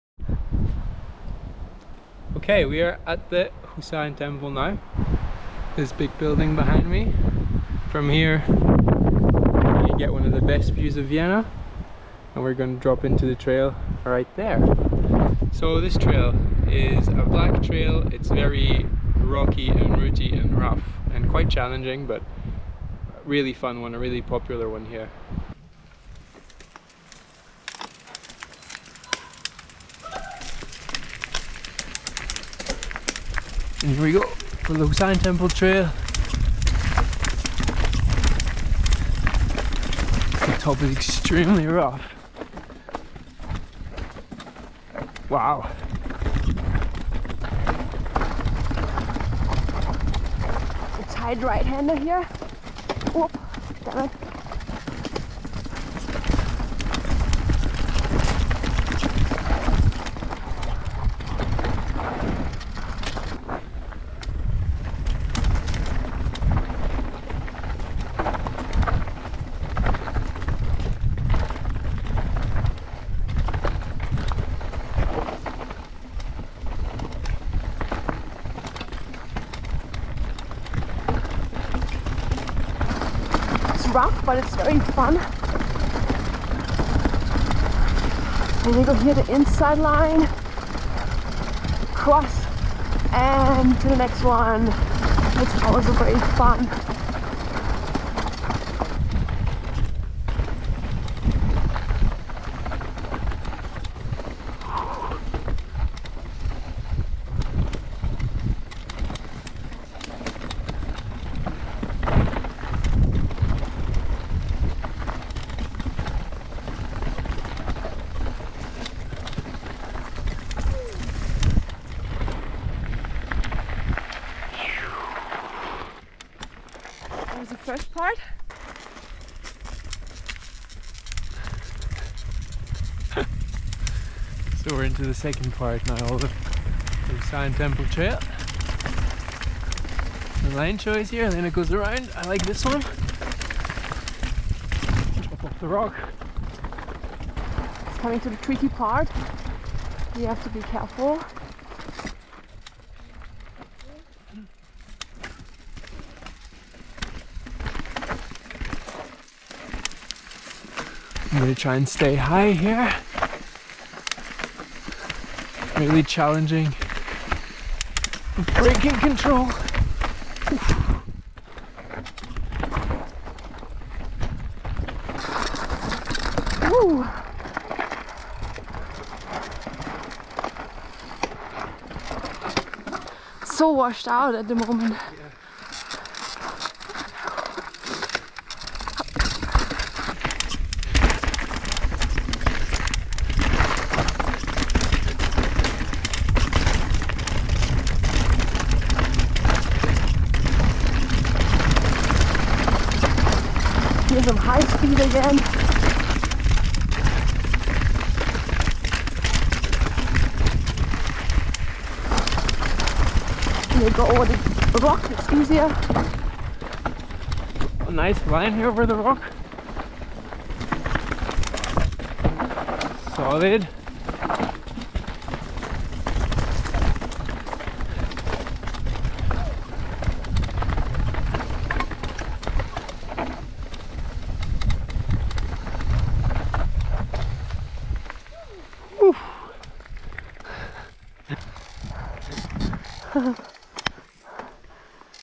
HUSARENTEMPEL TRAIL - Trail Preview
In this video, we preview the Husarentempel Trail, located on the Anninger, in the Wienerwald.
It’s a black graded trail, rough and rocky, but a classic here. The views from the top are amazing.

Riders: Helene Fruhwirth and Alex Chapignac
Supported by Radon Bikes, Picture Clothing, Sweet Protection and GOPRO

Recorded with a Gopro Max and Go Pro 11. Mounted directly on the helmet and with a chest mount.